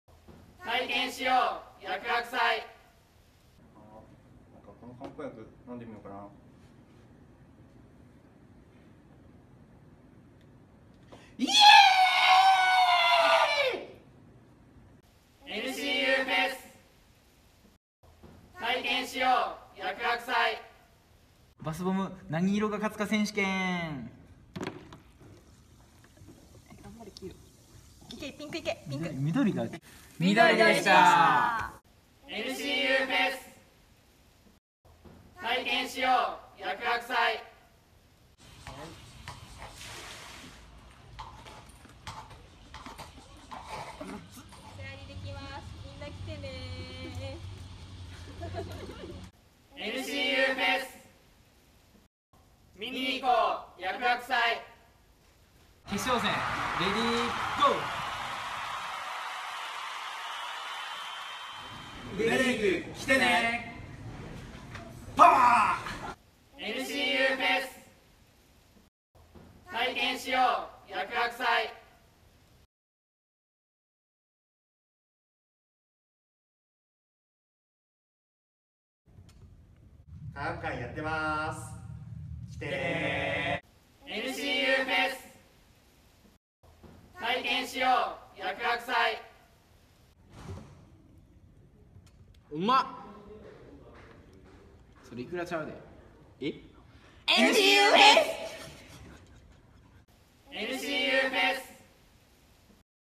ncu fes.2017 薬学祭ＣＭ集（名古屋市立大学 大学祭）
10月21・22日に瑞穂区の田辺通キャンパスで行われる名古屋市立大学薬学祭のＣＭ（学生制作）を集めたものです。
今年で67回目を迎える薬学祭。テーマは「LINE〜繋がりは∞〜」です。学生や来場者らとの無限の繋がりを大切に、みんなで一つになれる大学祭を目指し、薬学部ならではの企画や楽しいイベントを用意しています。皆さんも∞の一員になりましょう！